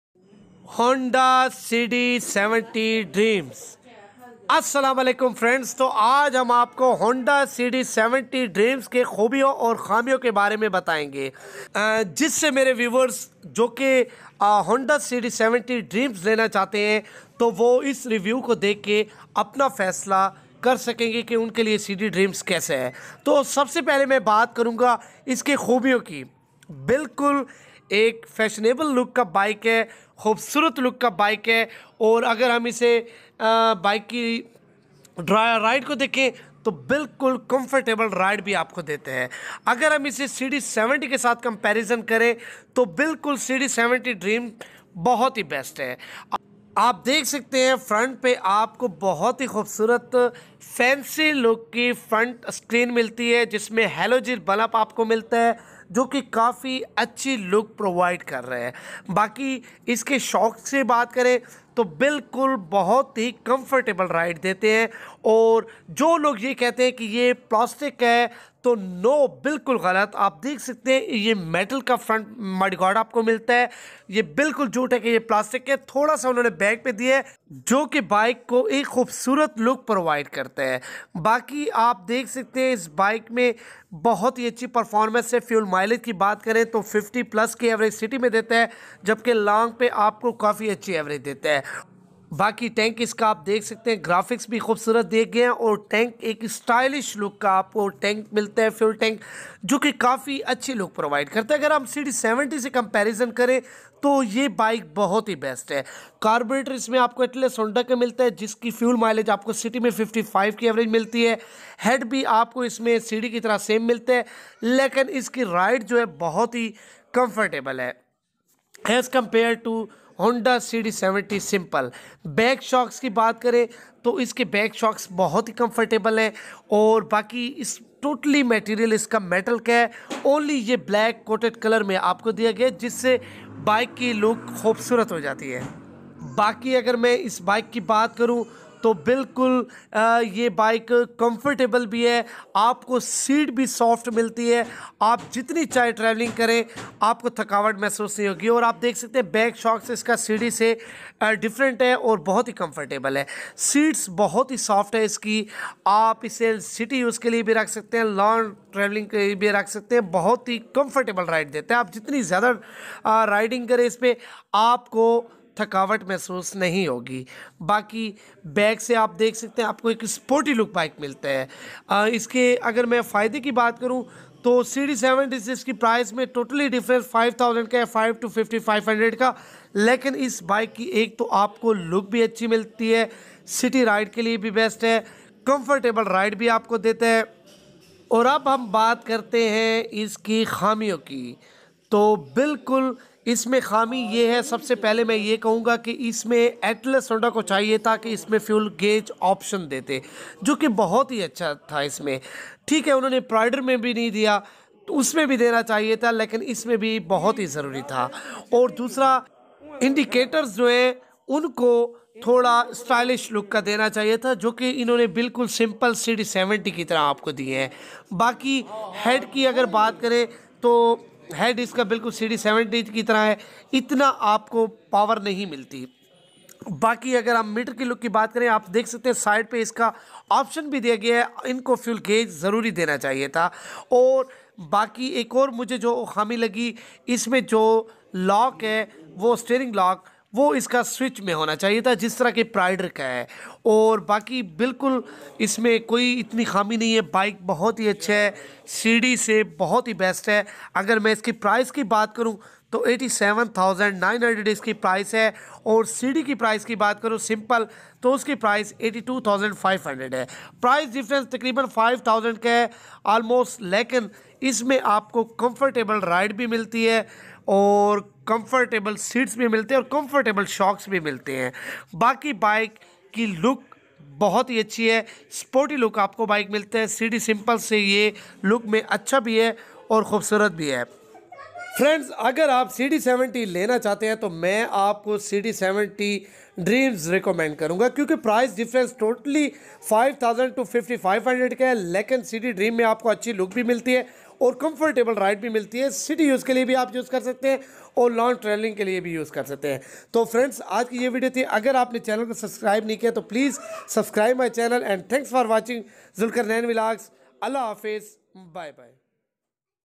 Honda Cd70 dreams | Watch this before buy a bike
honda cd70 dreams are good looking and fancy bike as compared to simple cd70.cd dreams shoks and ride are most comfortable,seats are soft best for city riding and long touring.